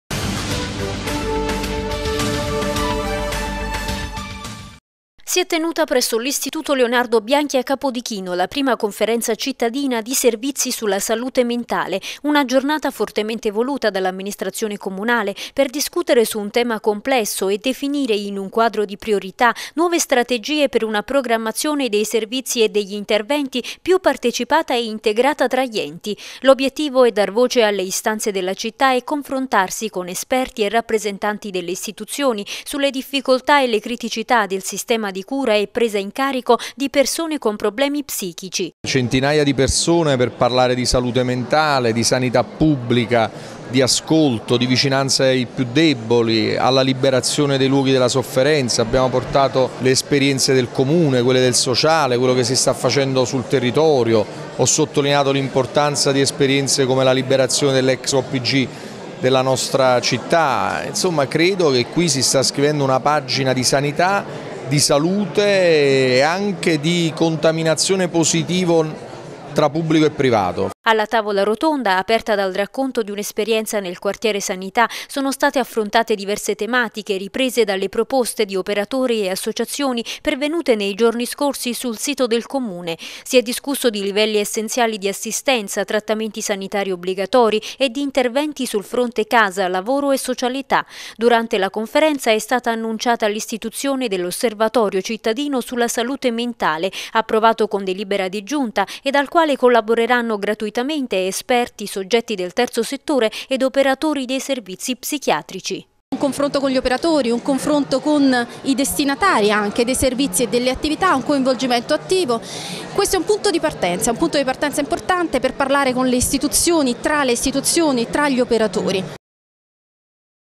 Salute mentale, verso una programmazione partecipata
All'Istituto Leonardo Bianchi la I Conferenza Cittadina di Servizi sulla Salute Mentale organizzata dal Comune. Un'occasione di dialogo e confronto per migliorare la programmazione dei servizi e degli inerventi.